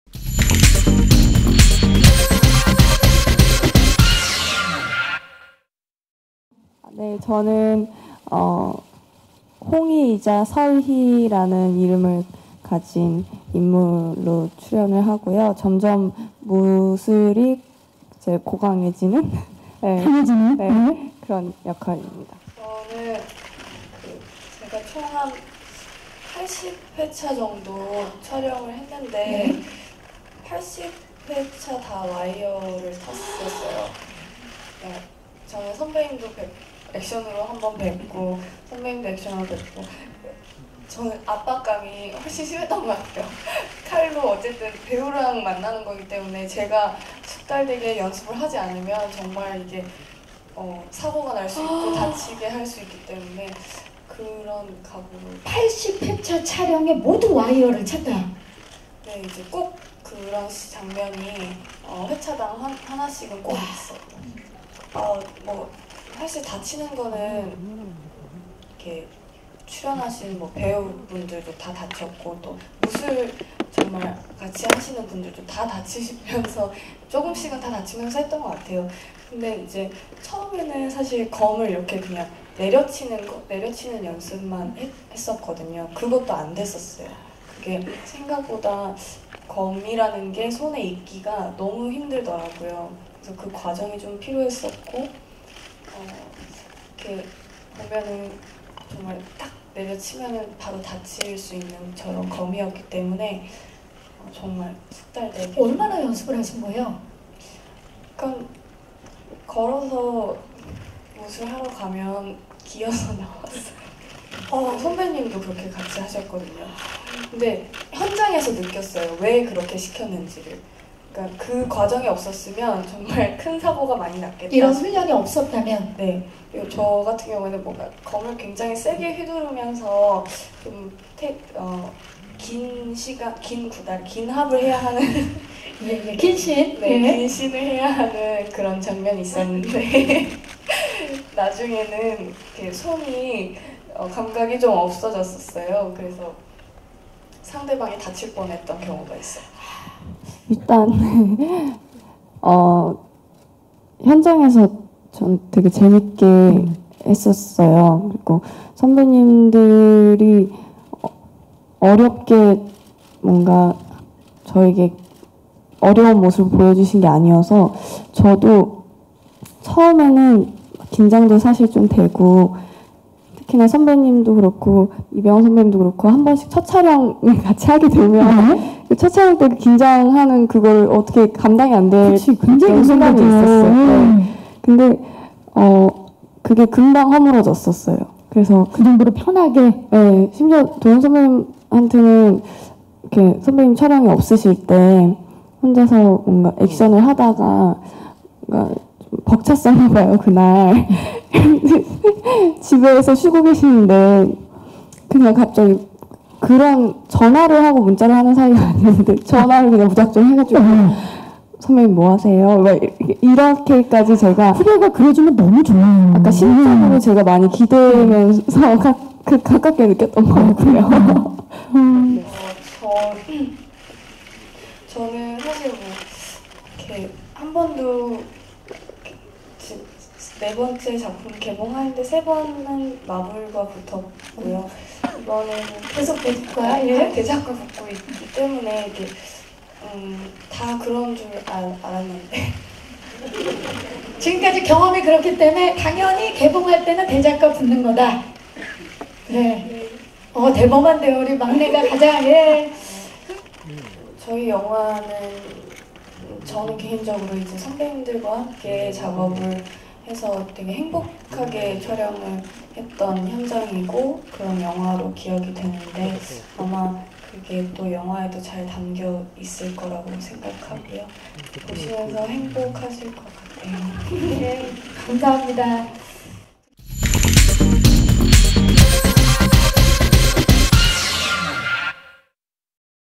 '협녀, 칼의 기억' 제작보고회 - 김고은 '질의응답'
[S영상] ‘협녀, 칼의 기억’ 김고은 “무술 연습, 걸어 들어가 기어 나왔다”

[스타데일리뉴스=박은희 기자] 김고은이 무술 연습을 혹독하게 한 사연을 털어놨다.

영화 ‘협녀, 칼의 기억’ 제작보고회가 24일 오전 11시 서울 광진구 자양동 롯대시네마 건대입구점에서 개최됐다. 이날 제작보고회에는 박흥식 감독과 이병헌, 전도연, 김고은이 참석했다.

김고은은 액션신에 대해 “내가 80회차 정도 촬영을 했는데 80회차 다 와이어를 탔다. 칼로 이병헌과 전도연이라는 배우랑 만나는 것이기 때문에 압박감이 컸다”라며 “연습을 열심히 하지 않으면 안됐다”라고 말했다.

그는 “배우들 모두 조금씩은 다 다치면서 했다. 처음에는 검을 내려치는 것도 안됐다. 생각보다 검이 손에 익는 게 너무 힘들었다”라며 “걸어서 무술을 하러 가면 기어서 나올 정도로 연습을 했다. 촬영현장에서 왜 그렇게 시켰는지 느꼈다. 그 과정이 없었으면 사고가 크게 났을 것”이라고 연습 과정을 설명했다.

- 후략 -

(영상 촬영 : 주성현 기자)